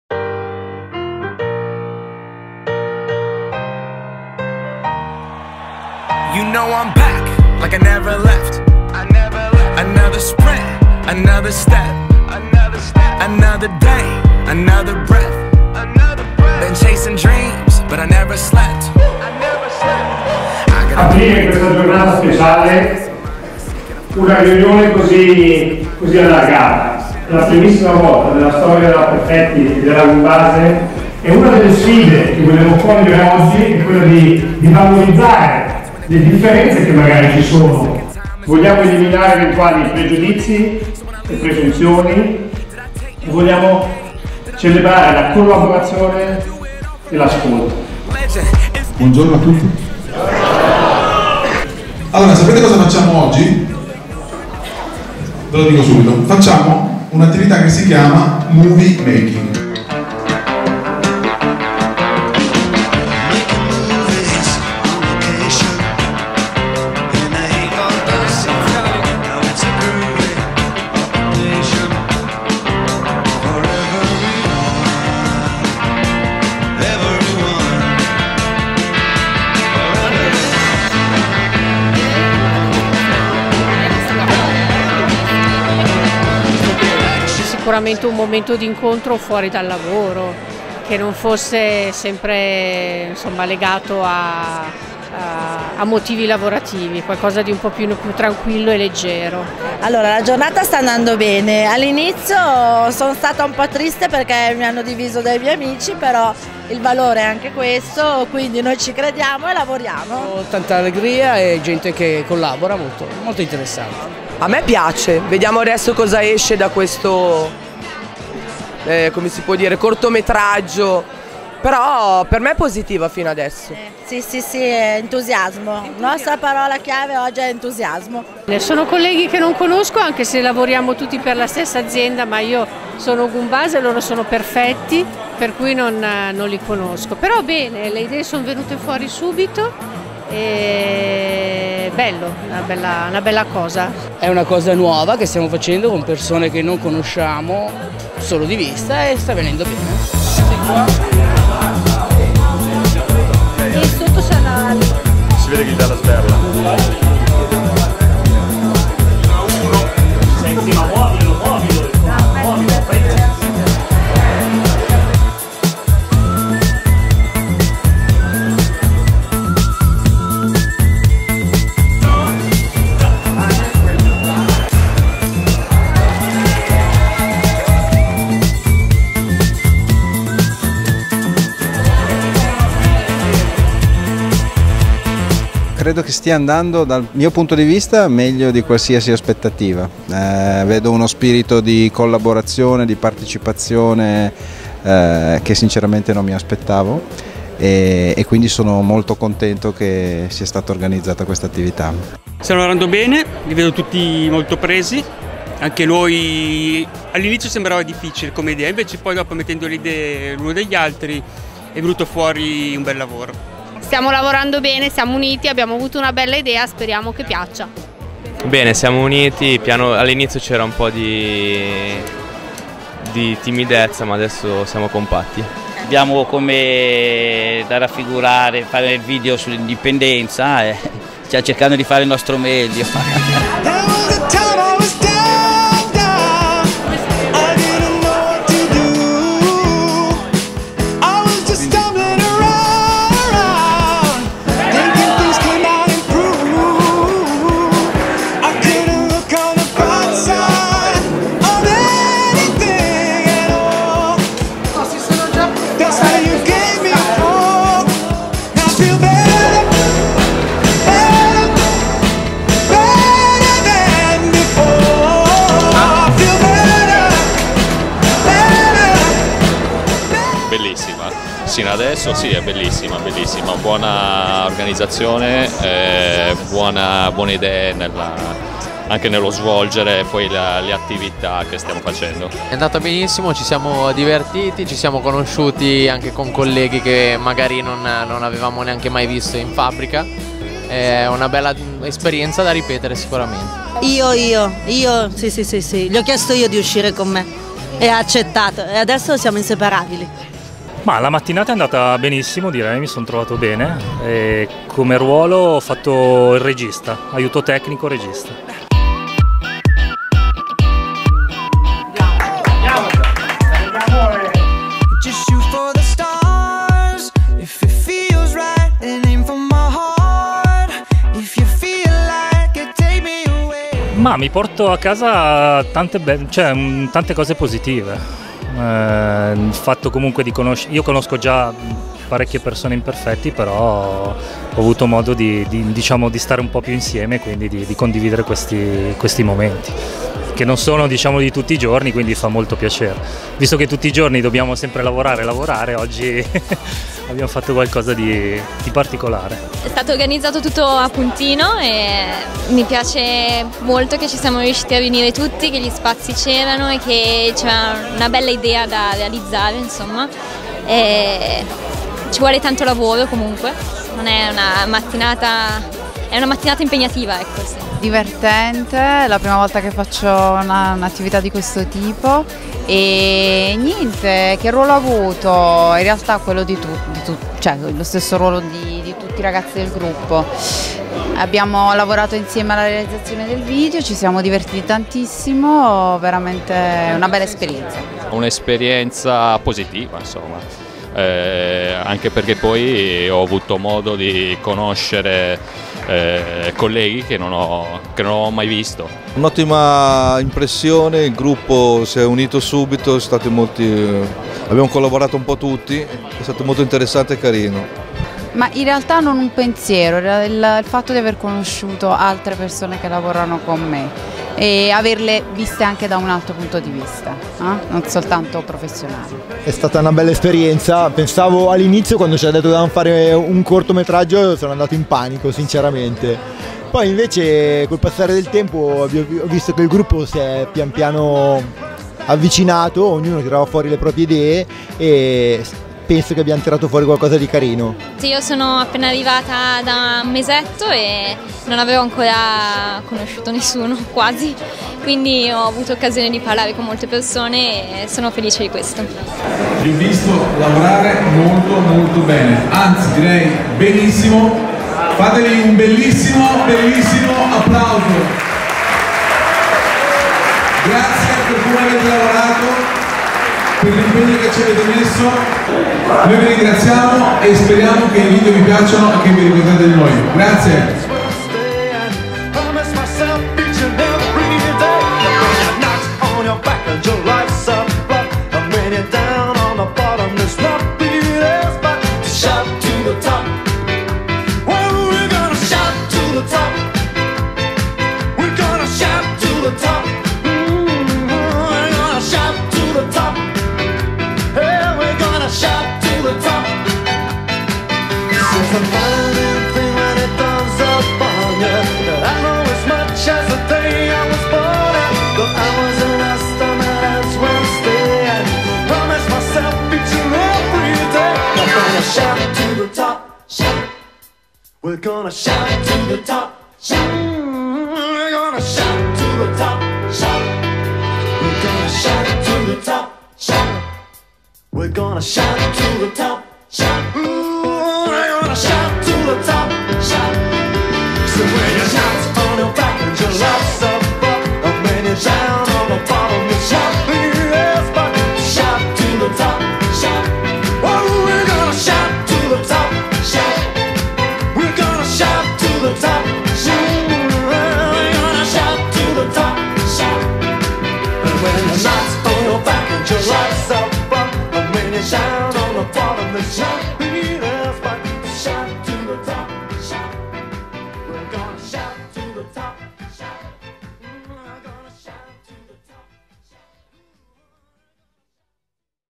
Movie Making Team Building
Team Building Movie Making: i manager dietro la cinepresa. Il team building Movie Making è un format creativo che può adattarsi a vari scopi di formazione o di animazione aziendale. Trasmette messaggi formativi d'impatto con uno stile serio o leggero, ironico e coinvolgente. Ma può anche rappresentare un'esperienza di puro divertimento.